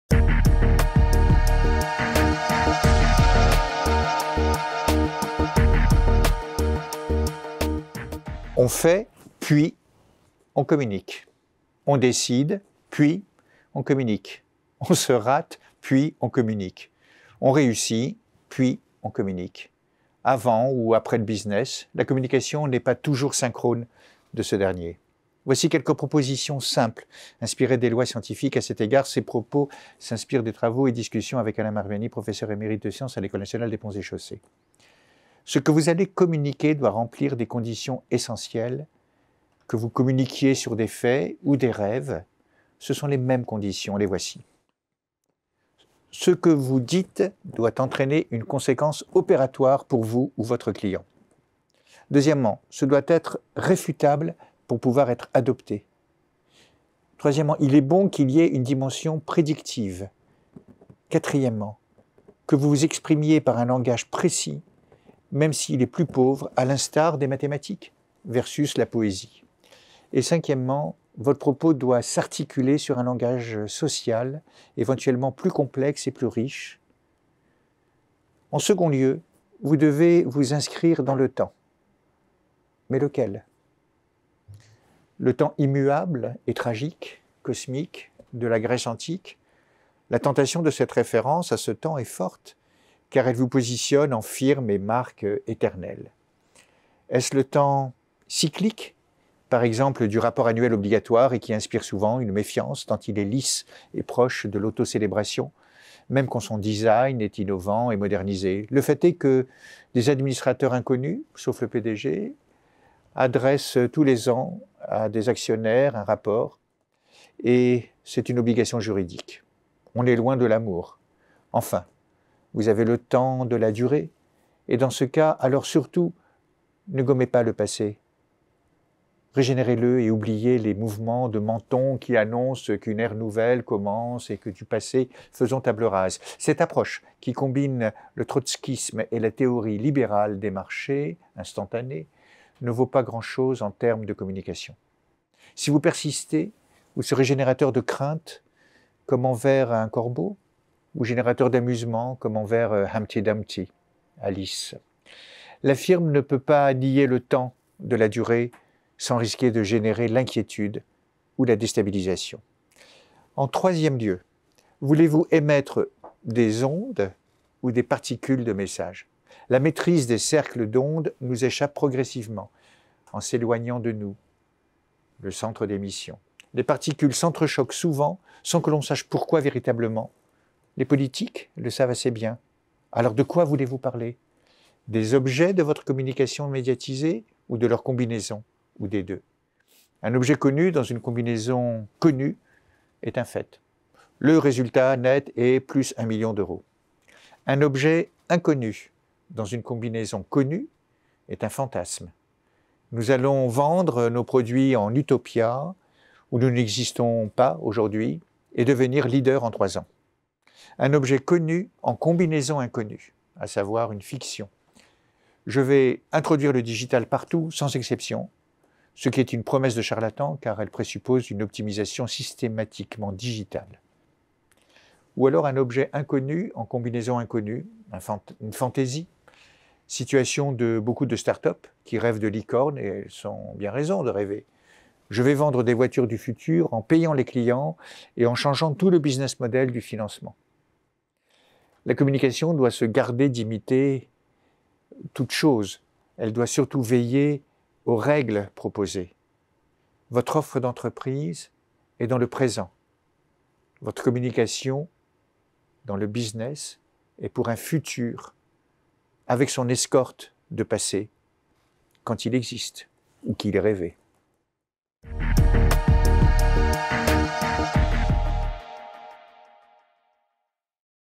Les conditions essentielles d'une bonne communication en business [Laurent Maruani]
On fait puis on communique, on décide puis on communique, on se rate puis on communique, on réussit puis on communique, avant ou après le business, la communication n’est pas toujours synchrone de ce dernier. [...]